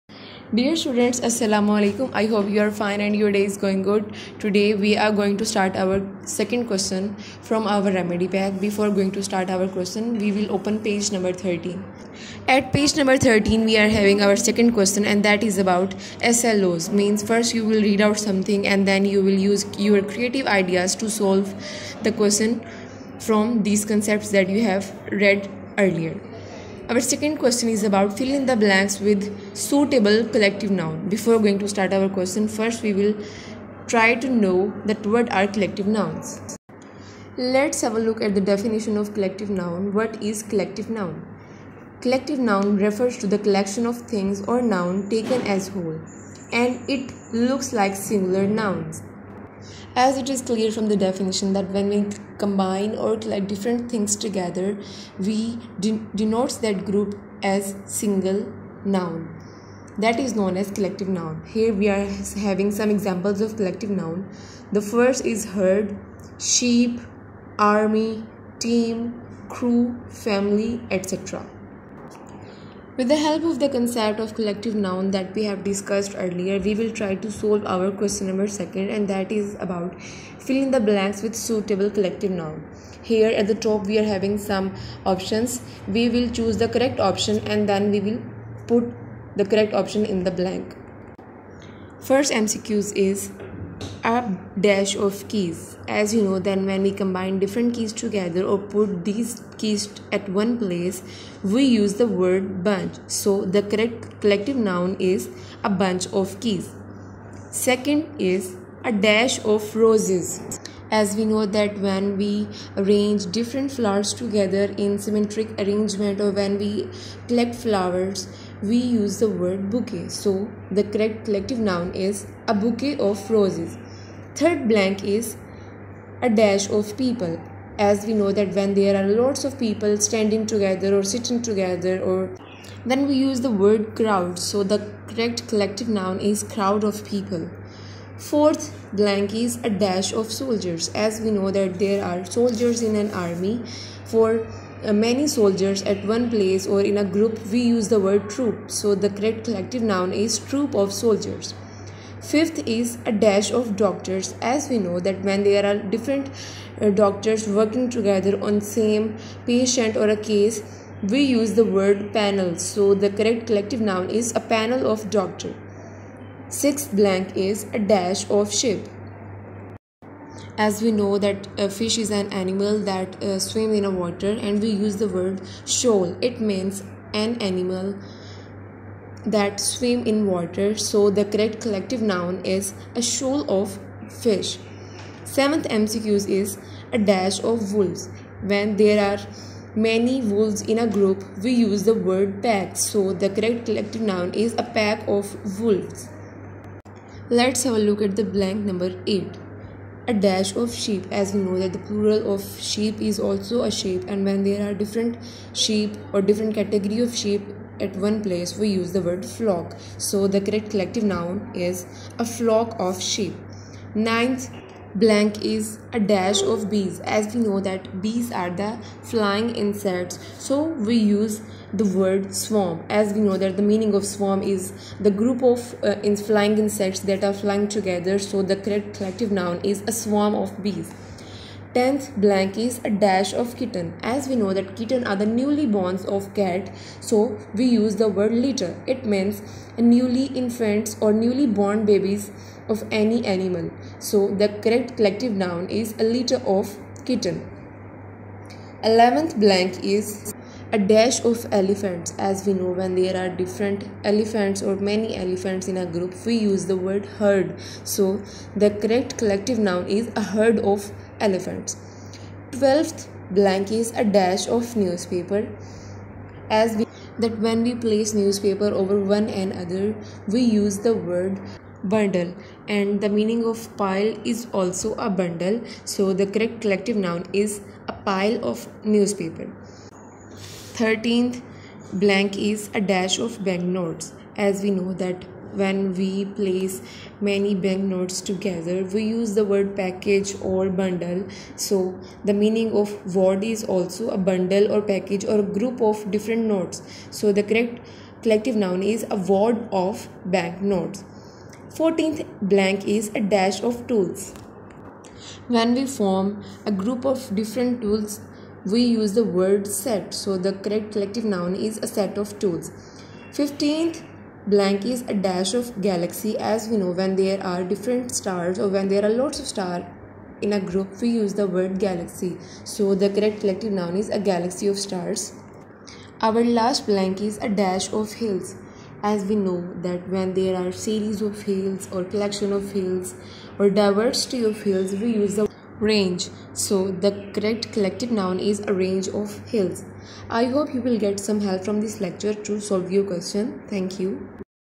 Level 7 II English II Remedy Pack Page 13
Dear students This video is about the level 7 English Remedy Pack Please Click the link below for the video you can complete your assignment after watching the video for more assistance please feel free to call .